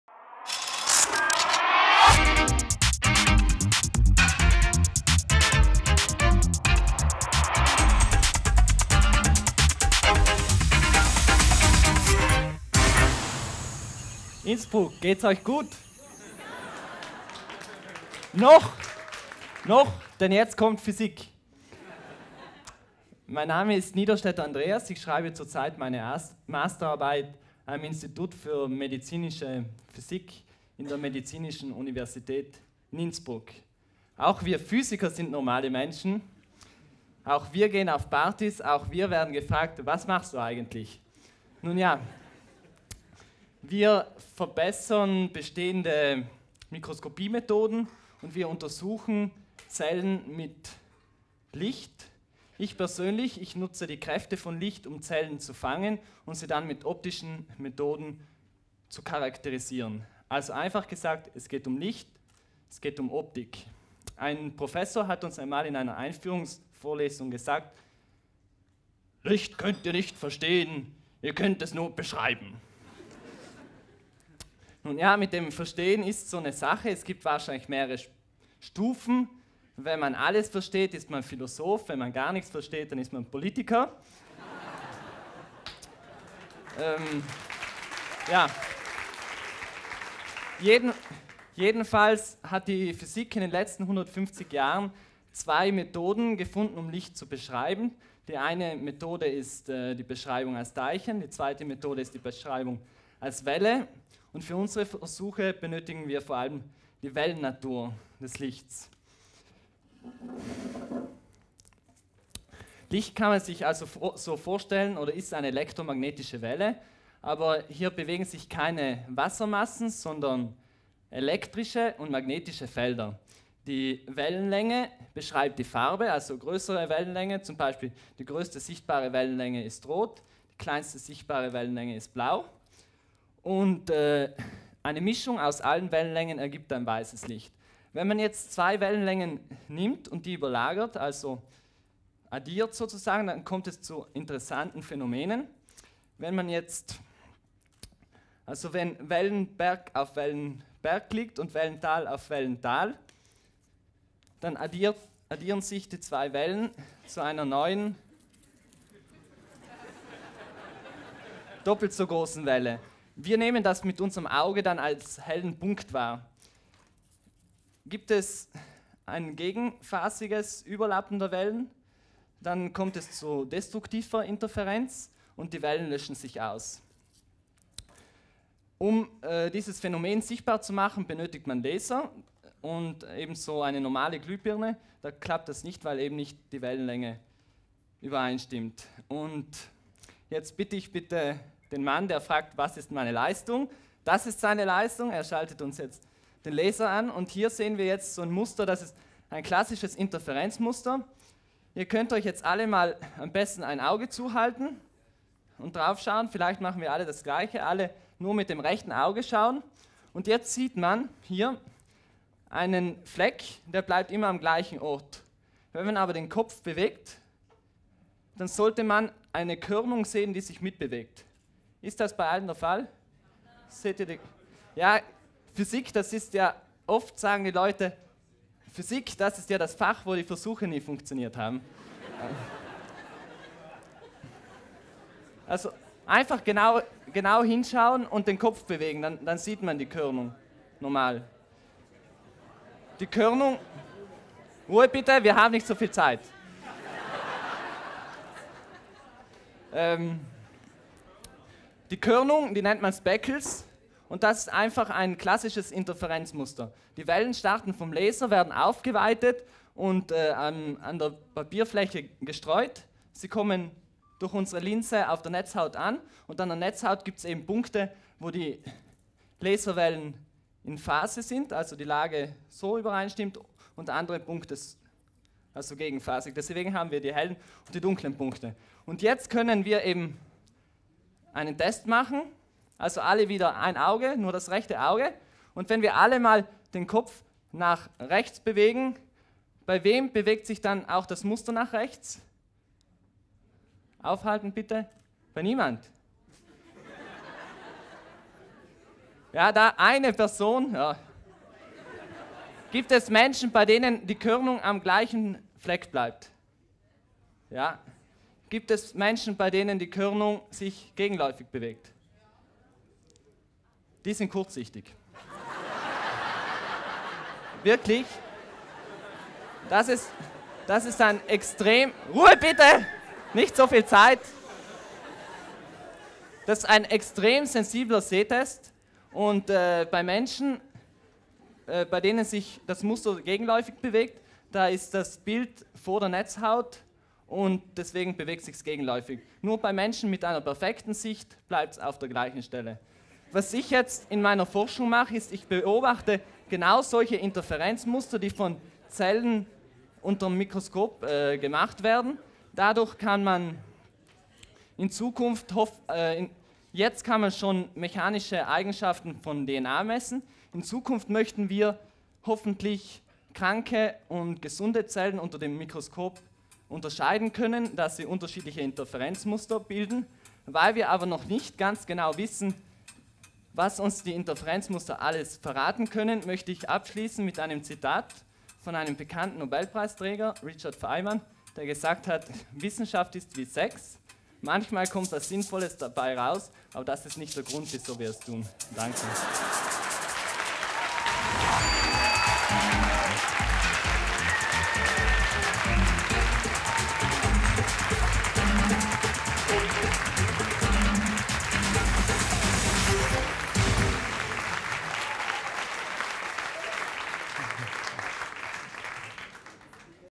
Andreas Niederstätter - Neue Laseranwendungen für Biologie und Medizin (Science Slam Innsbruck 2013)
2. Innsbrucker Science Slam Innsbruck. Aufgezeichnet am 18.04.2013 im Treibhaus Innsbruck.

Performers: Daniel Rigo, Rita-Maria Neyer, Claudia Paganini, Gerald Zernig, Andreas Niederstätter und Michael Kohlegger

Aufzeichnung und Postproduktion:
Alpsolut Moving Pictures

TV-Konzeptn:
media productions Film- und Medienproduktion GmbH